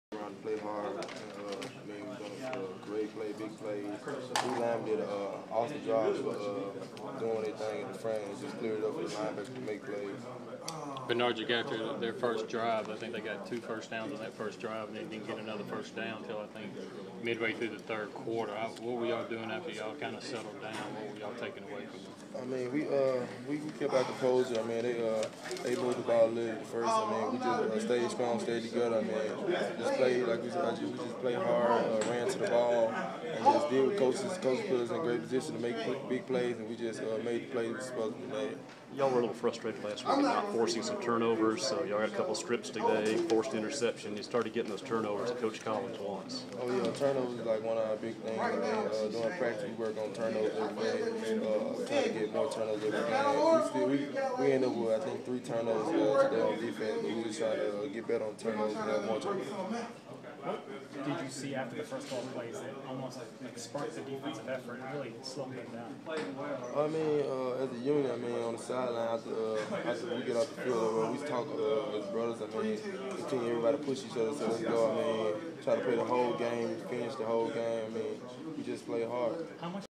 MSU sophomore LB Benardrick McKinney reacts to a 51-7 win over ASU
McKinney talks to reporters after the Bulldogs beat the Braves.